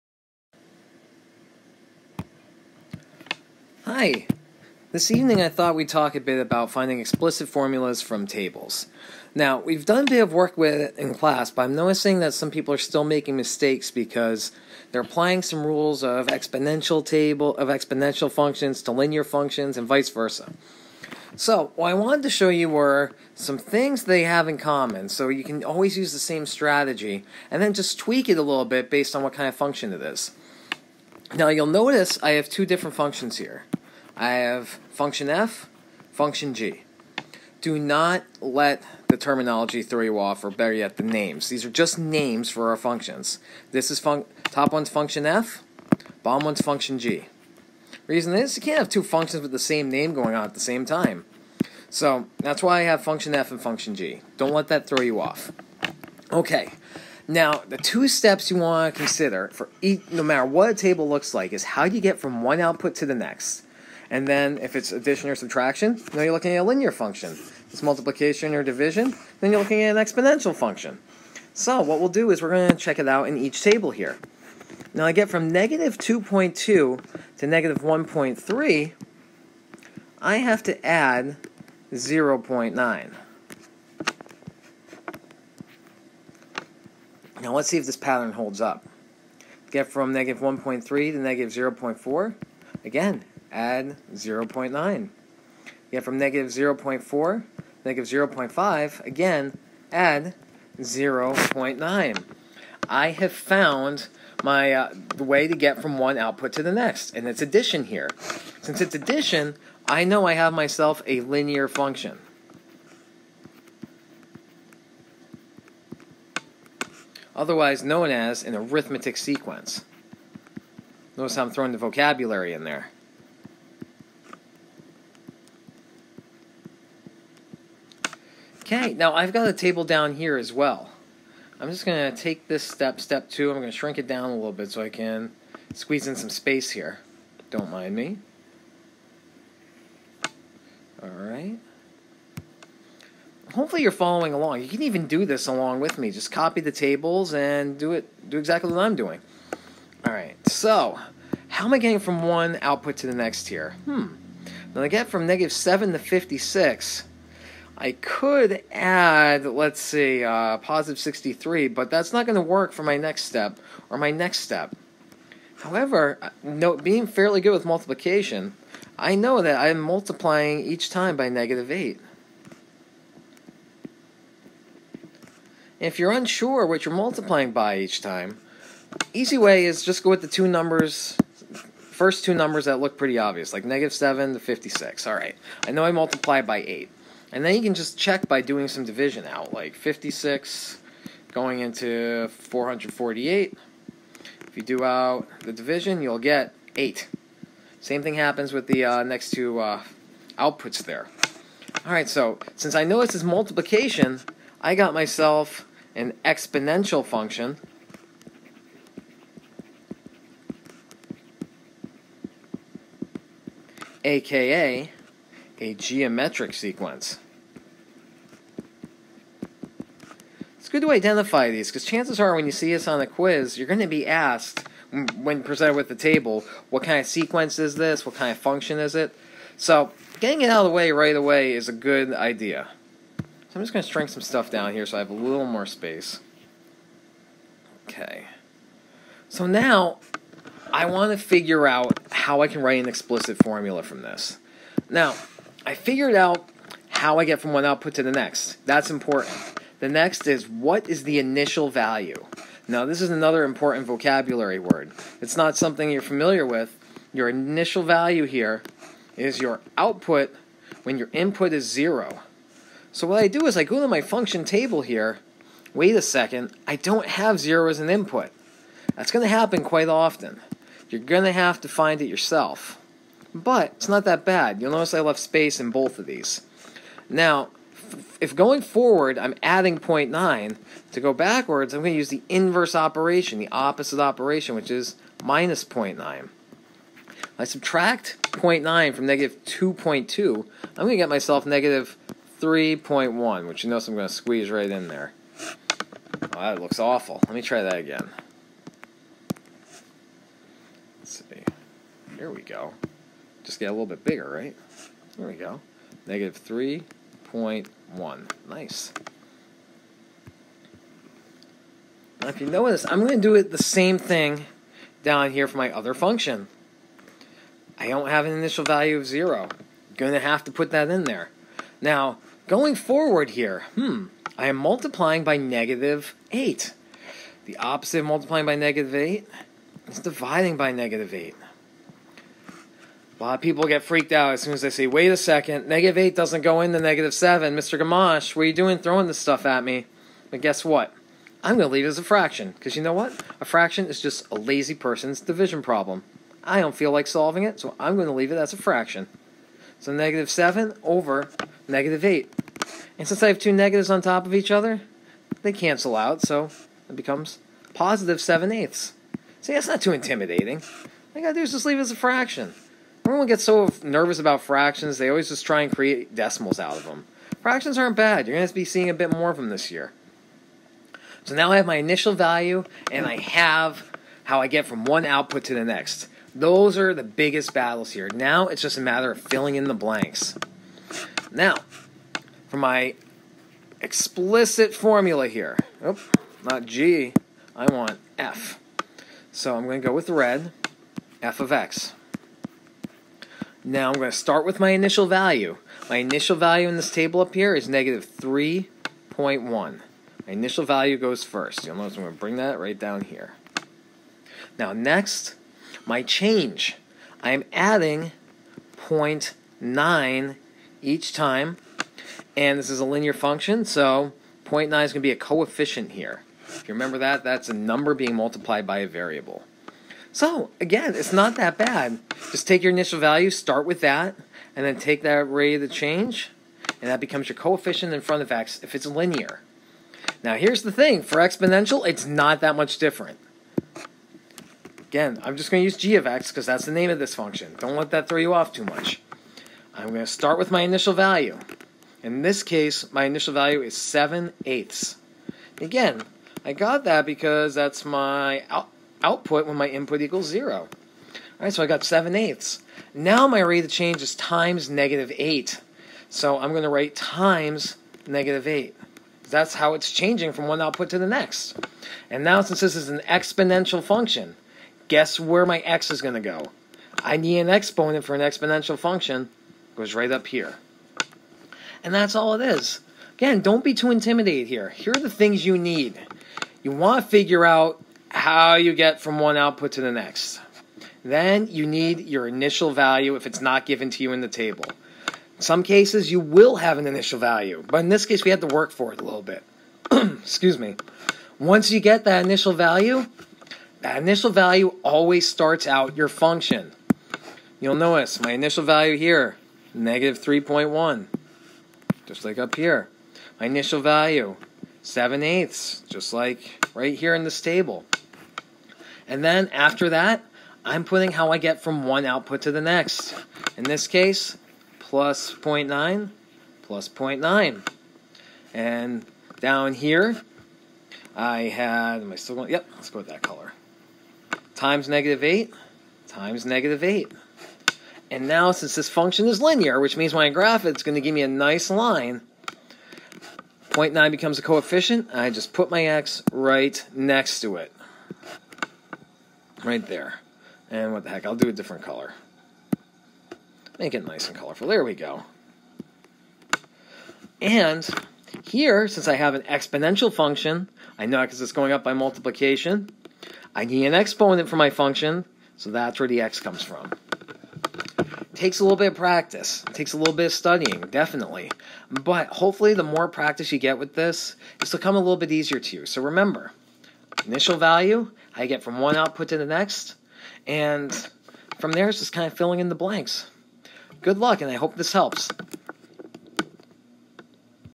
Explicit Formulas Tables (Linear and Exponential)
Mr. Gamache shows how explicit Formulas for linear and exponential functions are not that different. For both, find the initial value and how to get from one output to the next.